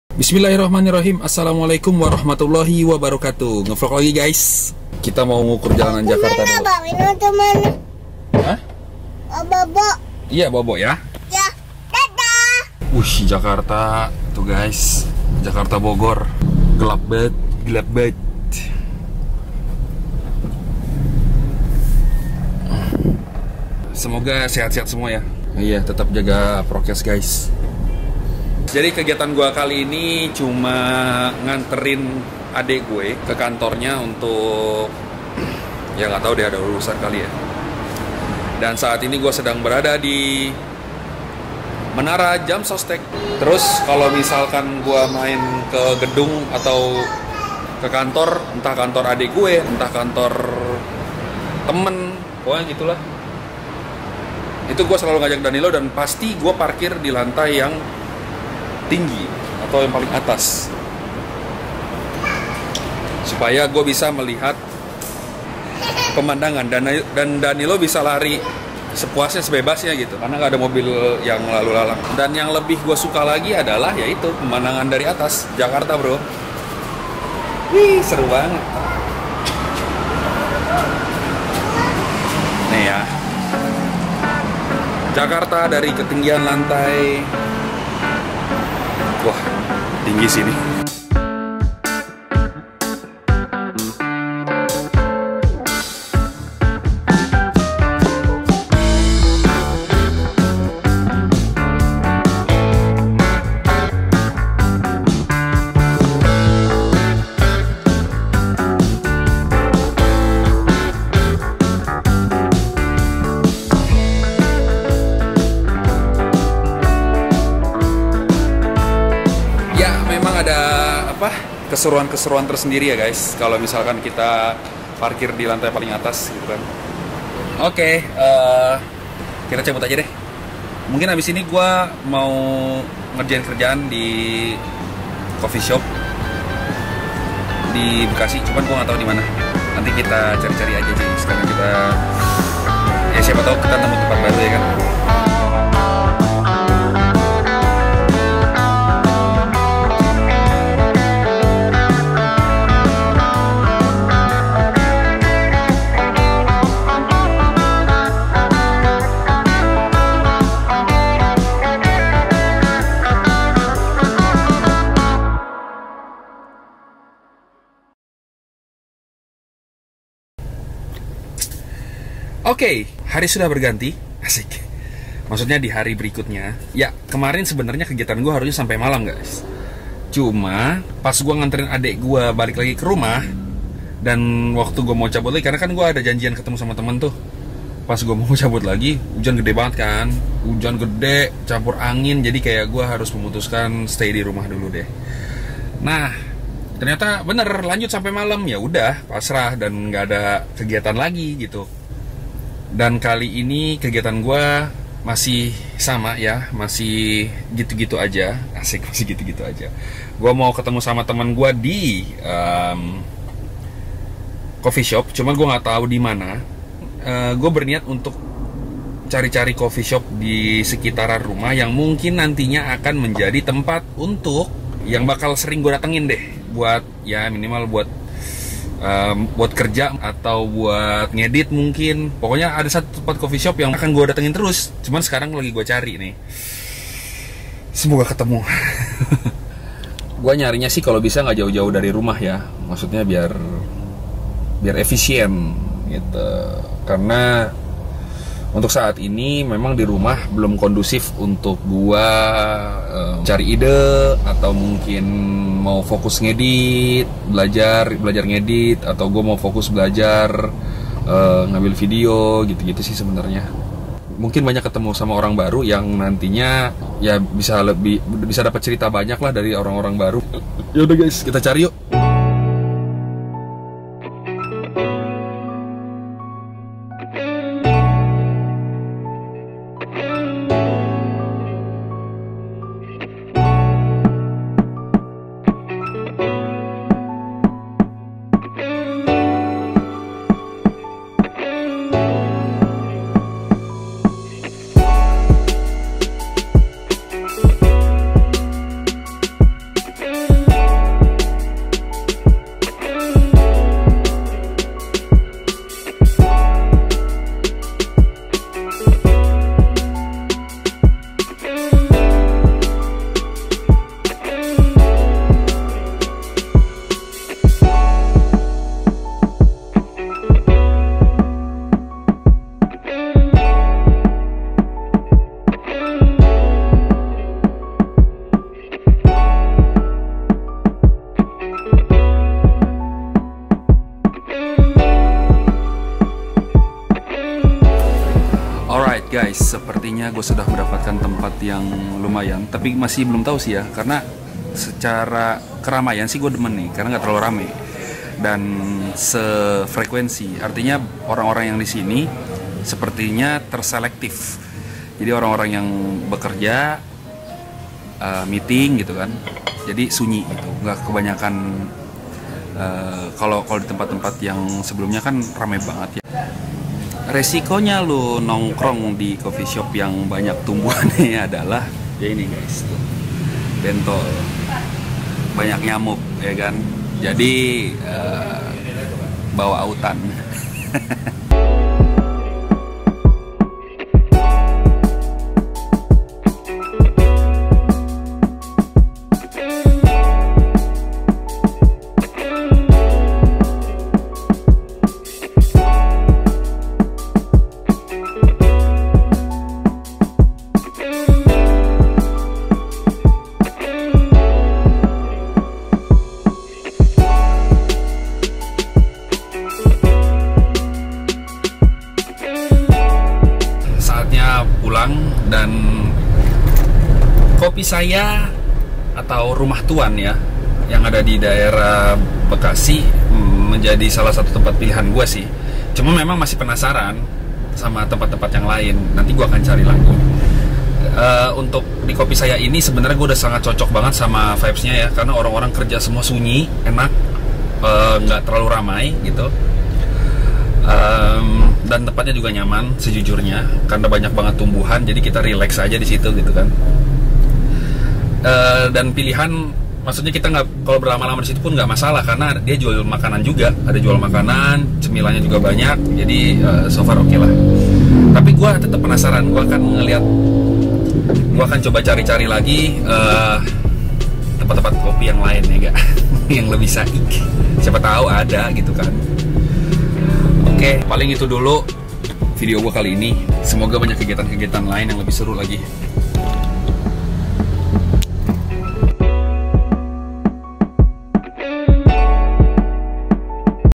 VLOG #37 | Menara Jamsostek, Coffee Shop Saya di Bekasi dan Sebuah Misi | DailyVlog
assalamualaikum,

kembali di video gw yang lainnya, kali ini gw ga ada kegiatan sama sekali ahahaha, tapi akhirnya gw diminta untuk anterin adek gw ke kantor nya. lumayan lah ada content wkwk.
dan next day nya, gw nyari coffeeshop yang kra kira cocok untuk gw pribadi di daerah deket rumah, nemu sih, tapi masih pengen nyari lagi.

penasaran ?? tonton terus video nya sampai habis,
Dan Jangan lupa untuk Support selalu Channel ini agar selalu berkembang menjadi lebih baik dan lebih menghibur lagi.